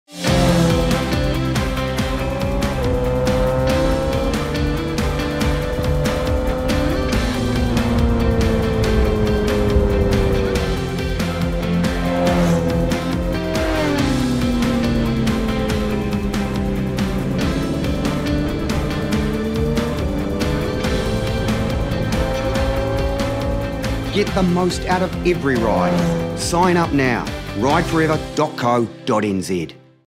Ride Forever - Great rides deserve great riders
New Zealand has some of the most scenic rides in the world. To get the most enjoyment from riding them, you need to have great on-road skills.

Ride Forever can give all motorcyclists the skills to make every ride a great ride.

Find out more about our course options to suit your riding experience